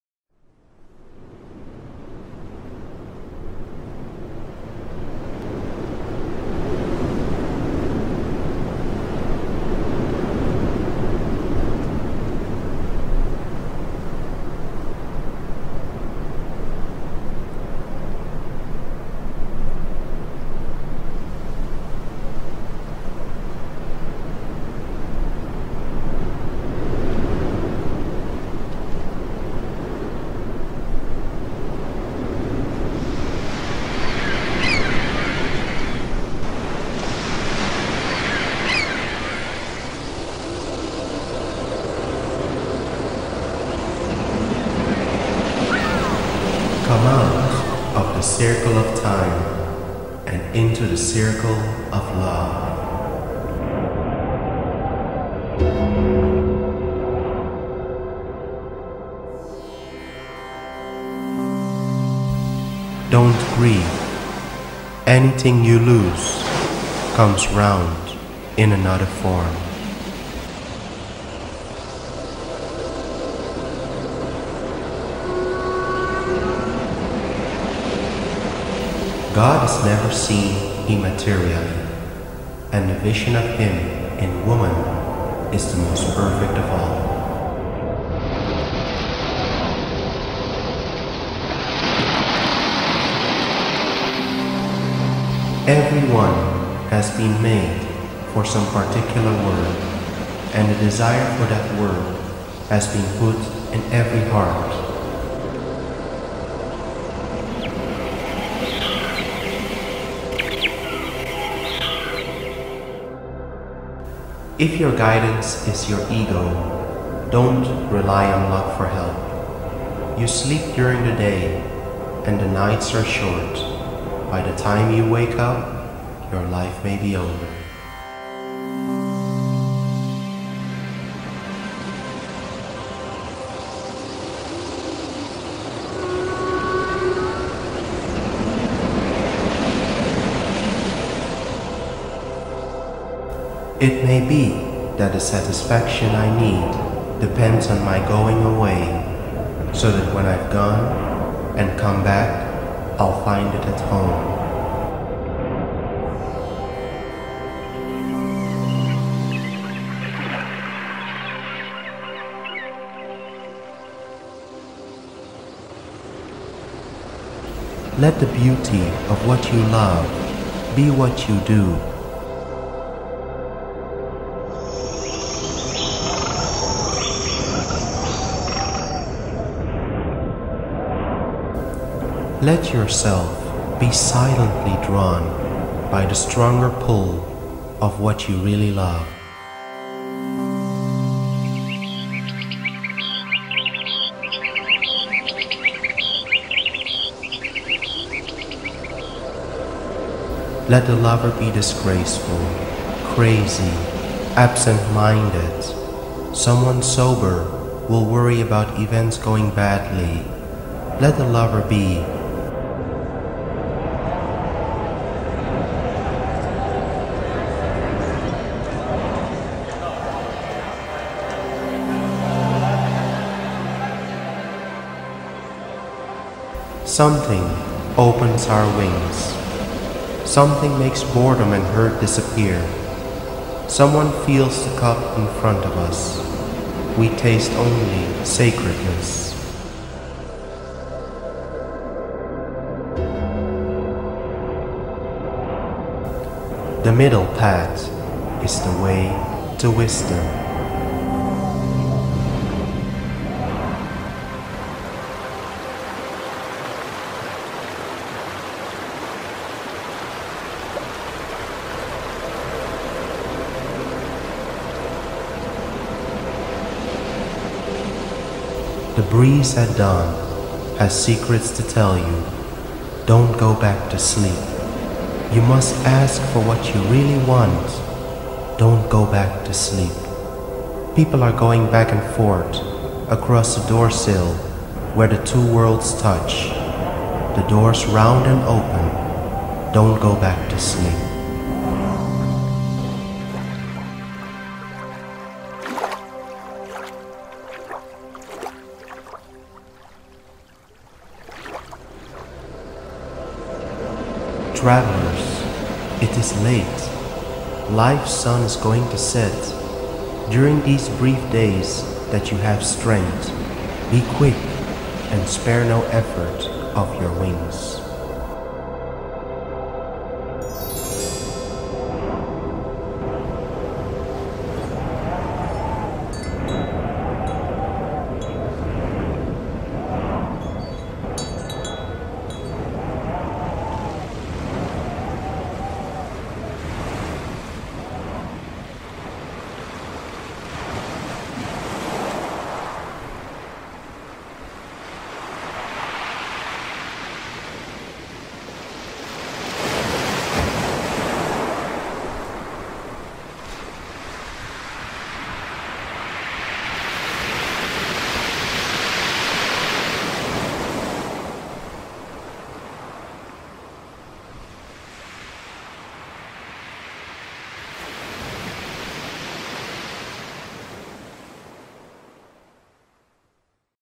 OH GOD THY SEA IS SO BIG AND MY BOAT SO SMALL
Inspirational meditation video, which will help you to reconnect with your inner guidance

Poems of Jalāl ad-Dīn Rumi

Video design, narration and music by Paul Odafe Akpomukai